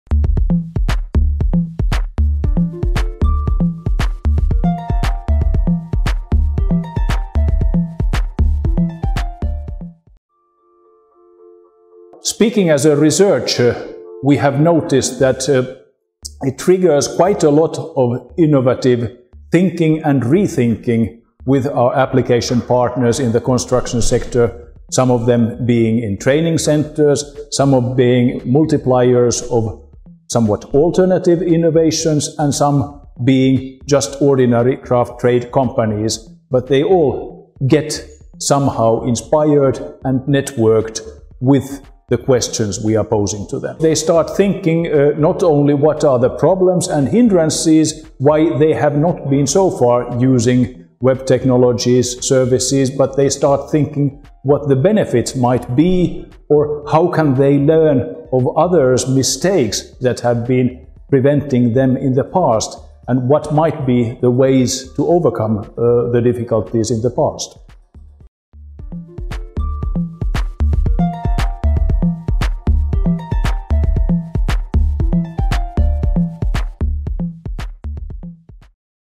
Pekka Kämäräinen - Why is LAYERS important?
- Pekka Kämäräinen tells us why he believes the Learning Layers is important